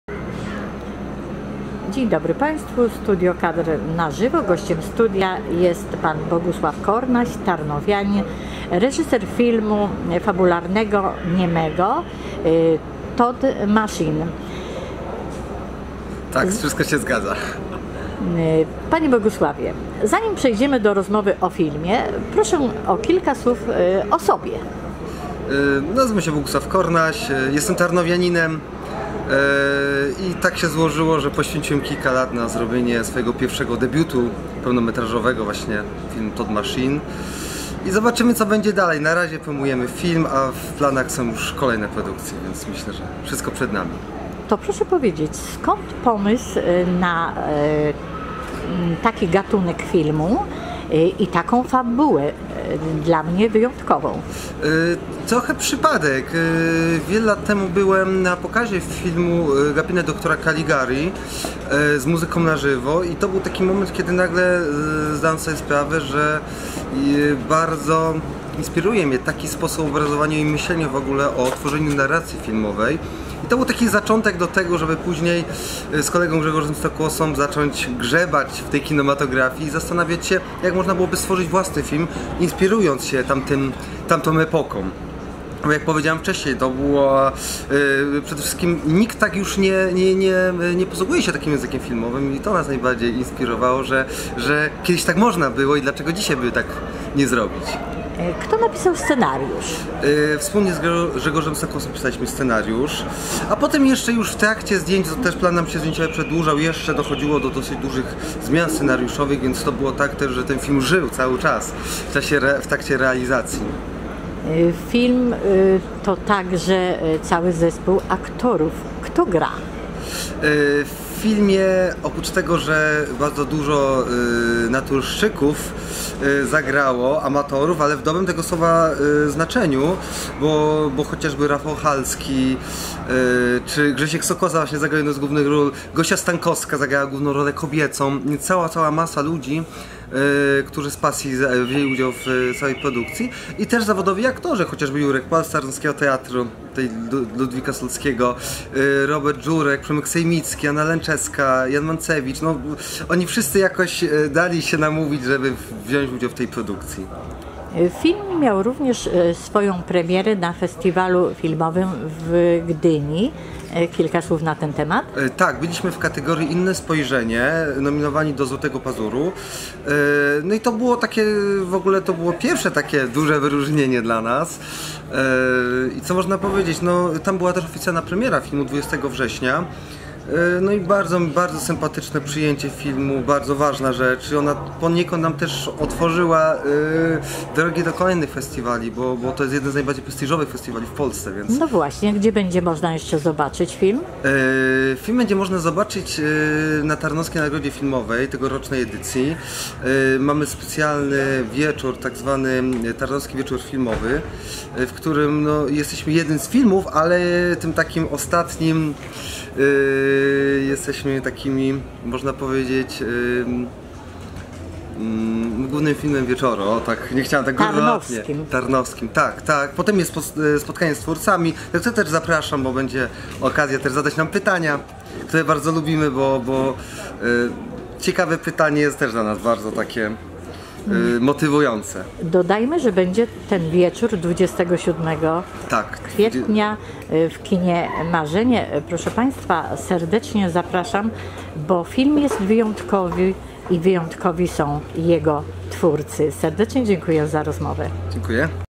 32. Tarnowska Nagroda Filmowa - TodMachine - Bogusław Kornaś
Tarnów 23 kwietnia 2018. Gościem KADRU jest BOGUSŁAW KORNAŚ, tarnowianin, młody, niezwykle kreatywny, reżyser pełnometrażowego filmu TodMachine.